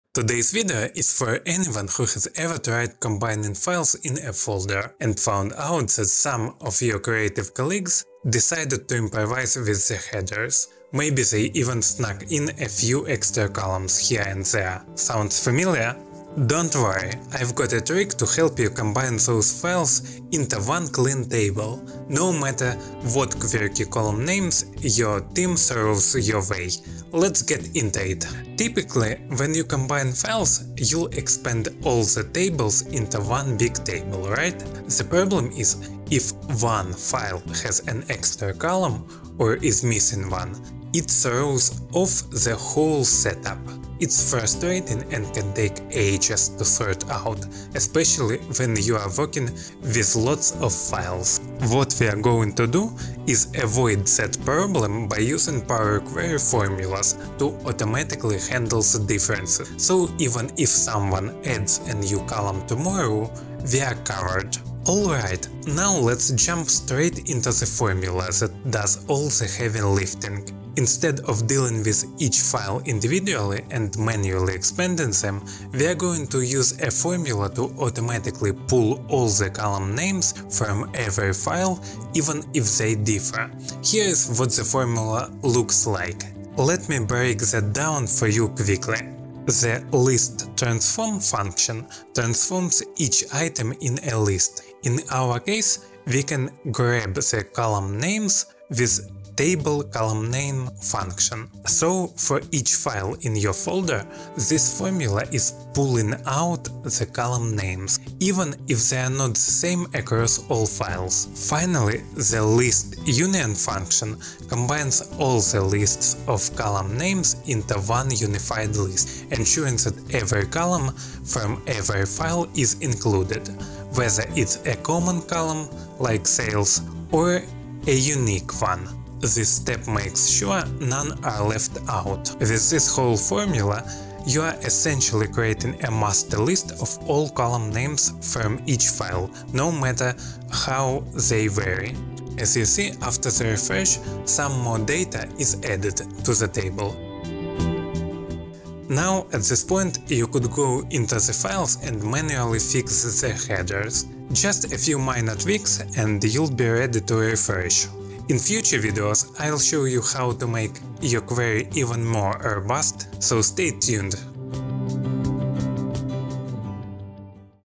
Power Query Trick: Combine Inconsistent Files Like a Pro
Combining files from a folder in Excel can be a real hassle when the headers don’t match or extra columns appear. In today’s tutorial, I’ll show you a simple Power Query method to solve this automatically. Instead of manually fixing each file, you’ll learn how to use a formula that brings in all your column names, no matter how they differ.

By the end of this video, you'll know how to:

    Handle different headers across multiple files.
    Combine files with varying numbers of columns.
    Build a complete list of column names from your folder.
    Make sure your query works even if someone adds new columns.

Power Query makes combining inconsistent files much easier and saves you time. Let’s get started.

Timestamps:
00:00 - Introduction: The issue with combining files with inconsistent headers
00:23 - Avoiding header mismatch problems
00:56 - Explanation of the formula: List.Transform and Table.ColumnNames
01:50 - The List.Union function
02:29 - What happens after the formula
02:39 - Final thoughts: Manual header fixes and future enhancements